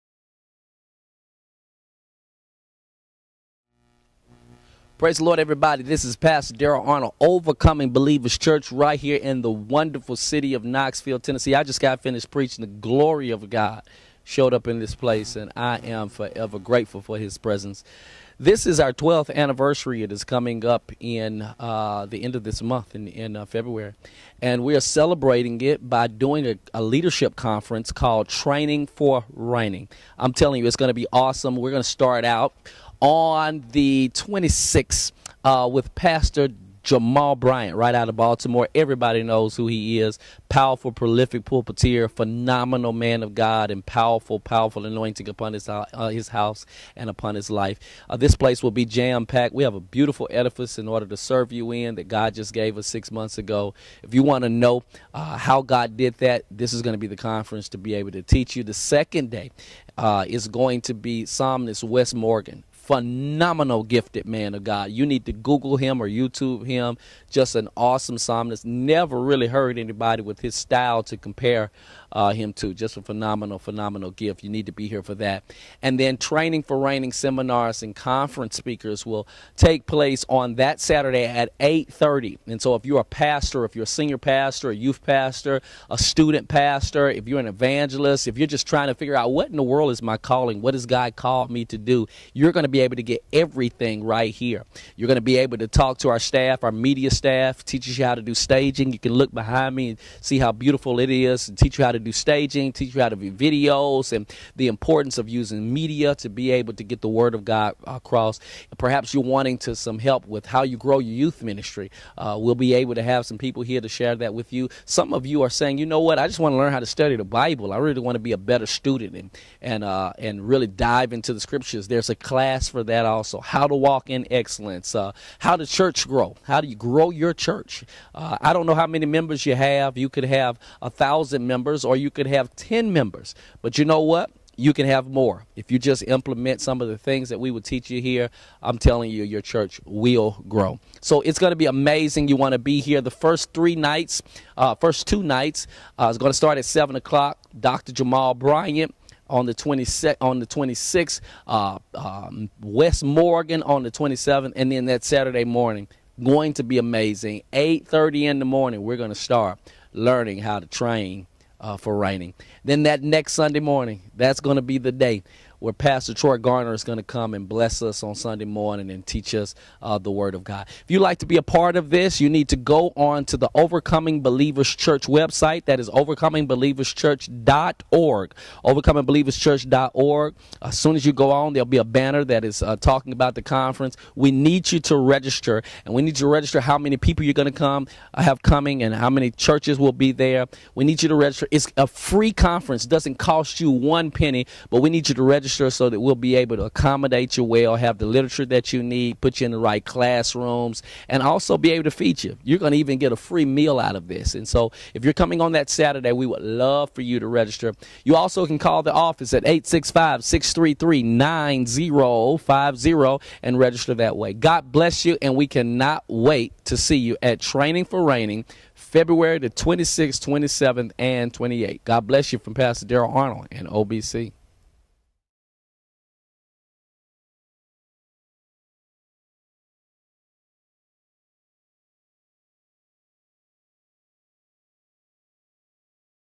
Training For Reigning Leadership Conference - Pastor Daryl Arnold
Training For Reigning Leadership Conference

Speakers:
Pastor Jamal Bryant, Empowerment Temple - Baltimore, MD
Pastor Wess Morgan, Celebration of Life Church - Hendersonville, TN
Pastor Daryl Arnold, Host - Overcoming Believers Church, Knoxville, TN

Homecoming Weekend:
THU 2/26, 7PM Dr. Jamal Bryant
FRI 2/27, 7PM Pastor Wess Morgan
SAT 2/28, 8:30AM - 4PM Training Sessions
SUN, 03/01, 12:30PM - 12th Anniversary Celebration